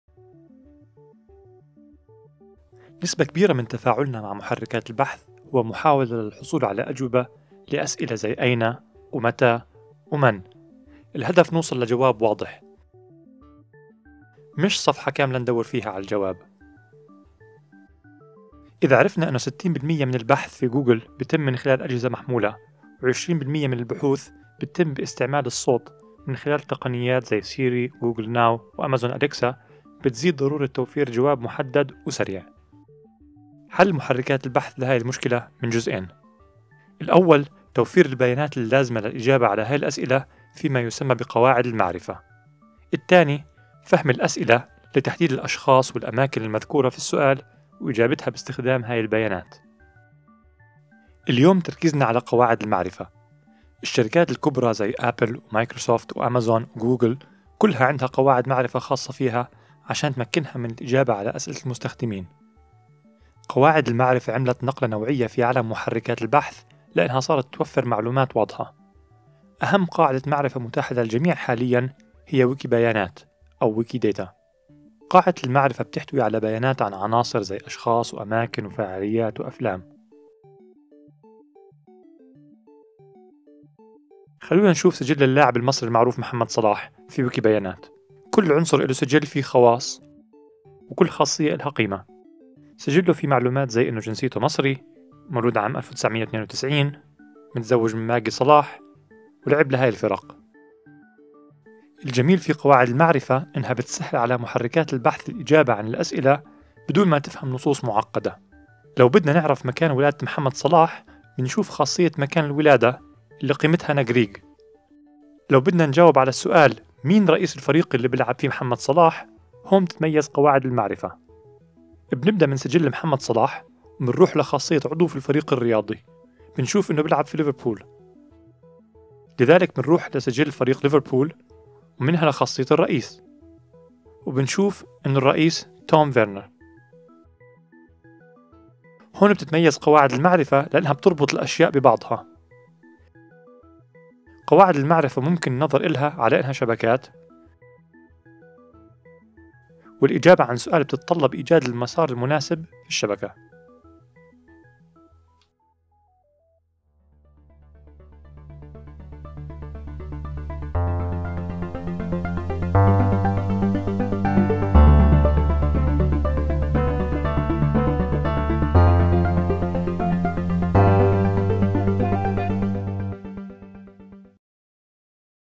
قواعد المعرفة | Knowledge Bases | العنصر 14 | الحلقة 9 | الموسم 1
قواعد المعرفة هي الخطوة الأولى لتمكين محركات البحث من توفير أجوبة واضحة على أسئلتنا وإجابة أسئلة معقدة. في هذه الحلقة نتعرف على قواعد معرفة، مستخدمين ويكي بيانات (أبرز قاعدة معرفة و مشروع شقيق لويكيبيديا) كمثال.

Knowledge bases are the first step in enabling search engines to give crisp answers to our questions. They also enable answering fairly complex questions. In this episode we look at knowledge bases, using Wikidata (the most prominent publicly available knowledge base, and a sister project of Wikipedia) as an example.

الحلقة مرفقة بالنّص المكتوب للكلام المحكيّ على الشاشة (closed captions).